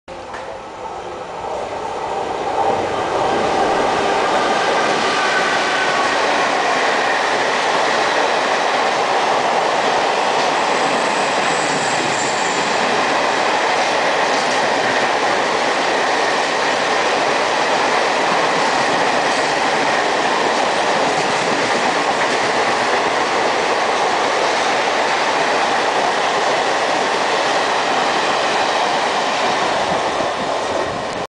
OFFLINK BRC WAM 4E # 21334 HAULED 4707 RANAKPUR EXPRESS
Bandra Bound 4707 Bikaner-Bandra T Ranakpur express Hauled by Offlink BRC WAM 4e #21334 loco and skipping through Bilimora jn station at morning sesson. (DUNGRI TRIP 25 JUL 09) Video taken by BHAVEEN PATEL IRFCA VADODARA